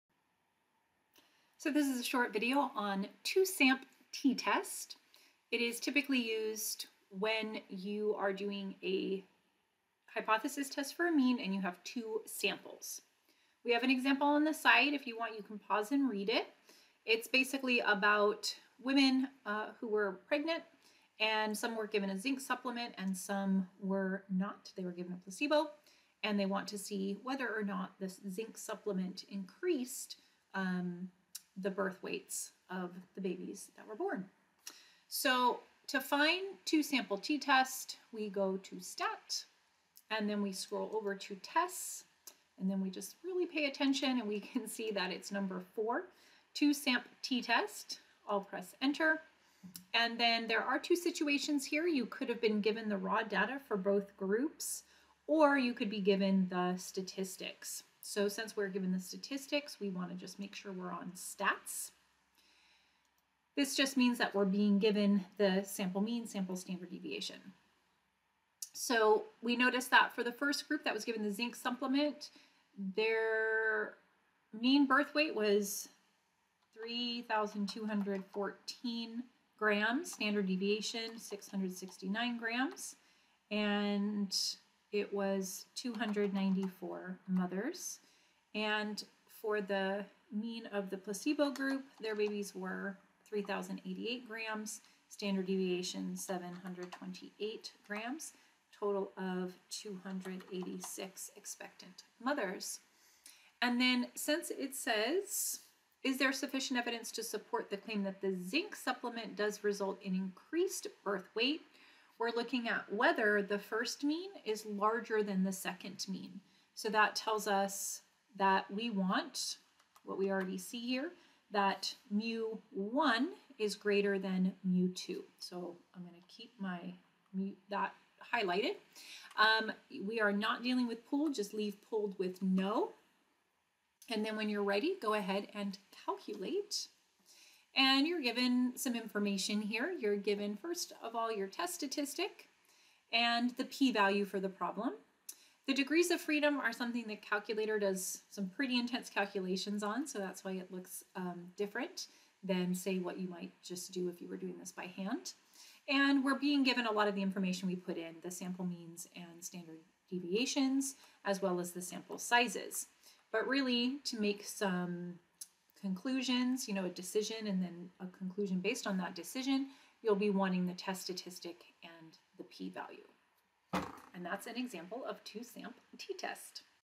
2 SampTTest example using the TI-84 Plus CE-T
In this video I show an example of using a graphing calculator to find important values for a hypothesis test involving the mean of two independent samples.